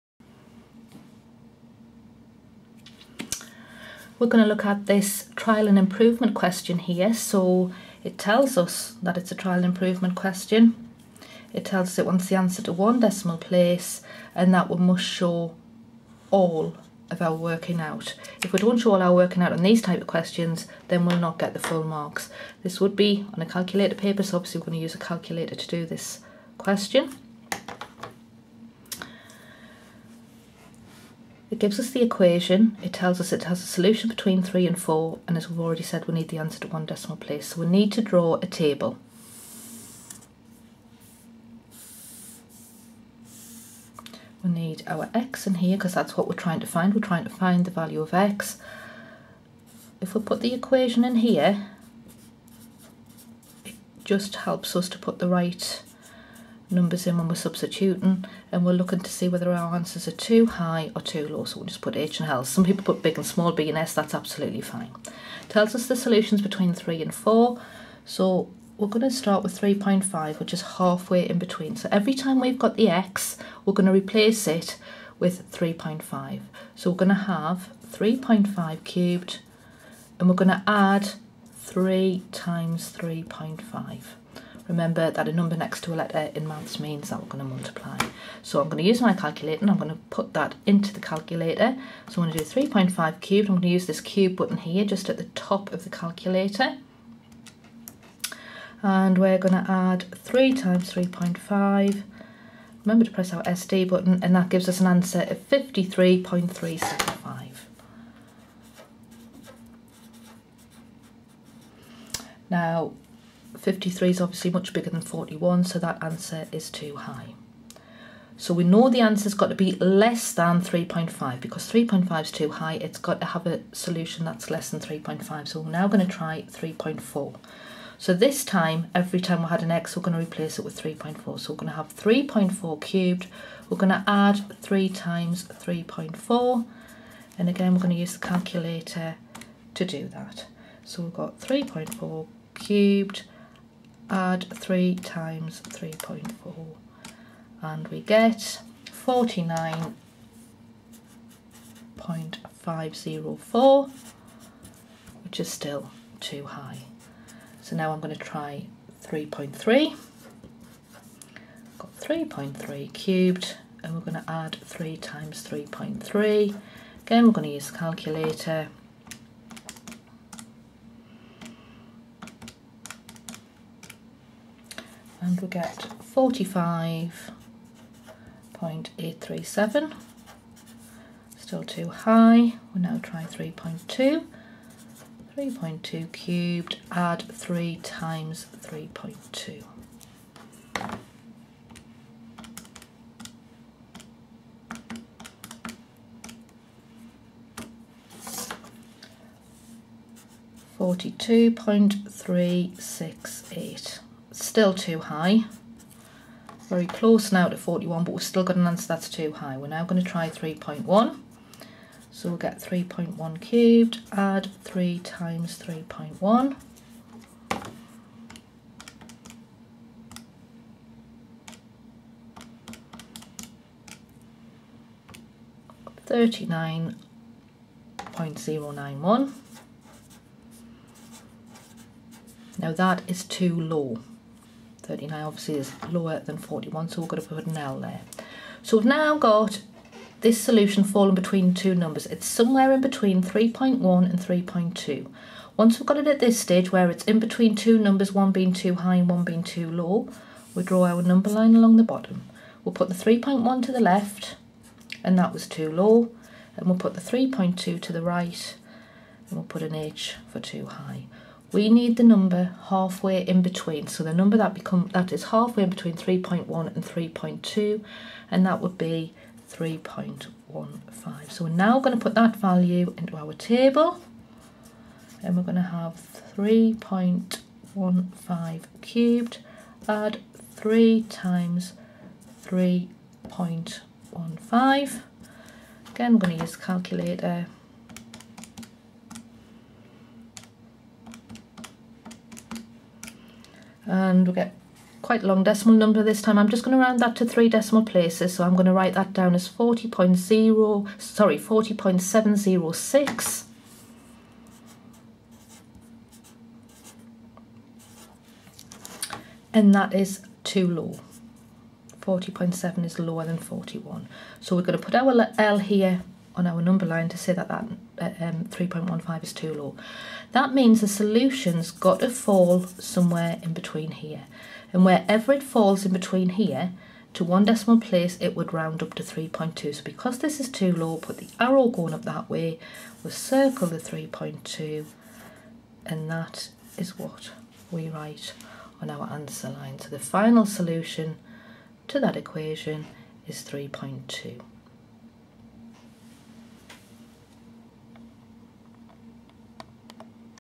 Trial and Improvement
How to answer a standard trial and improvement question from a typical GCSE exam.